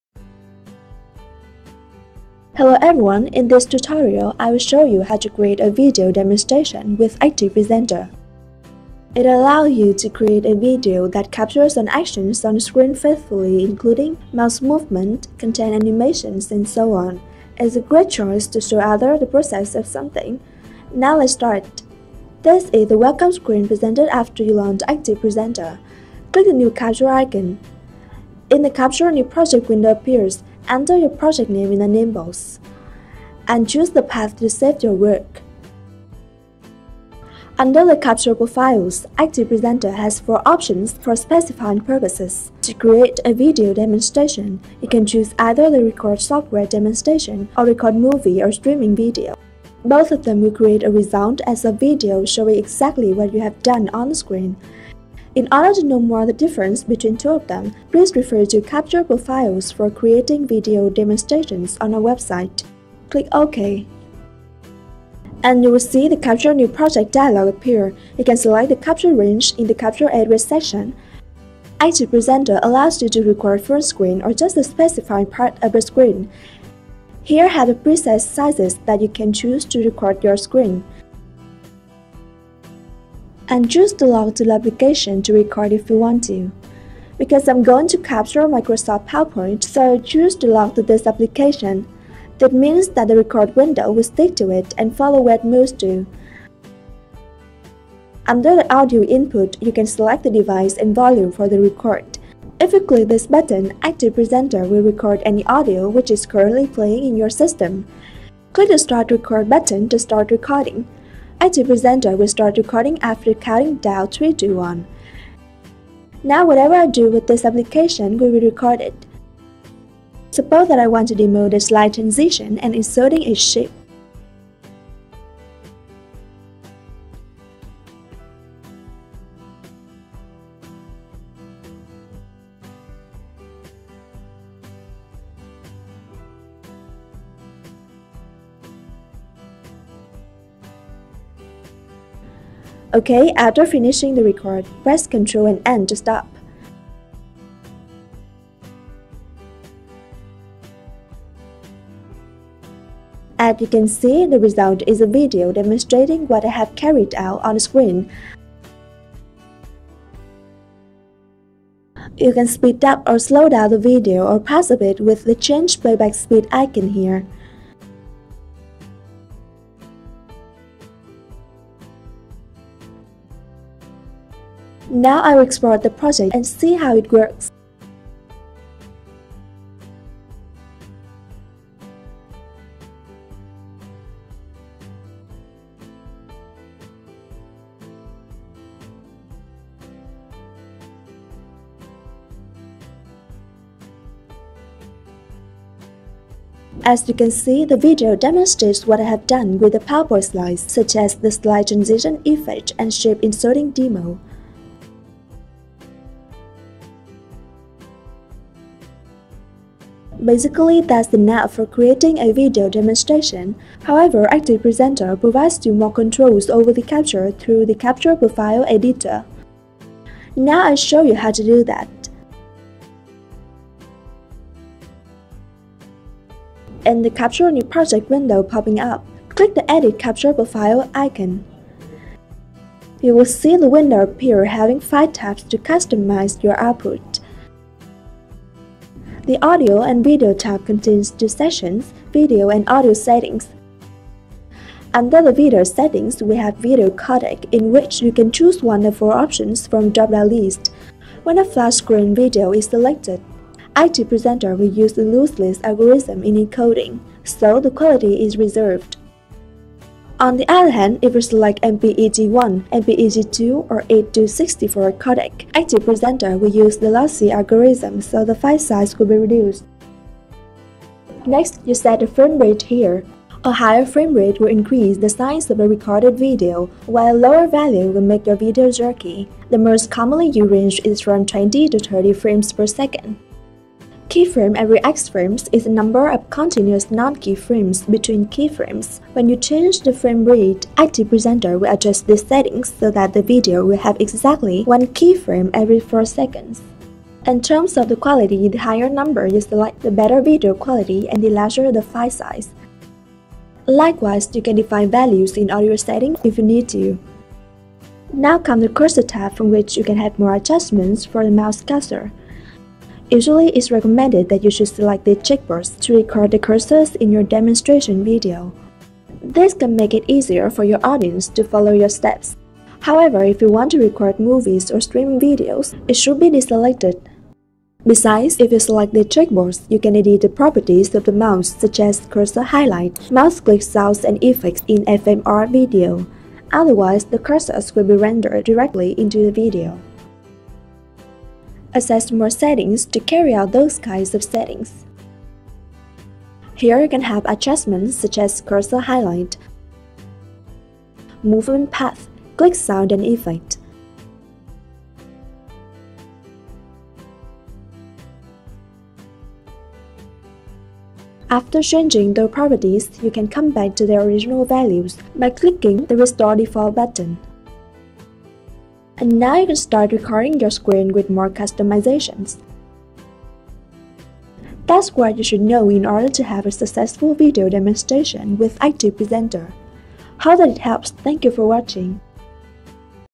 How to Create Video Demonstrations
In this video, we will learn how to create a video demonstration with ActivePresenter.

ActivePresenter - Screen Recording & Interactive eLearning Authoring Tool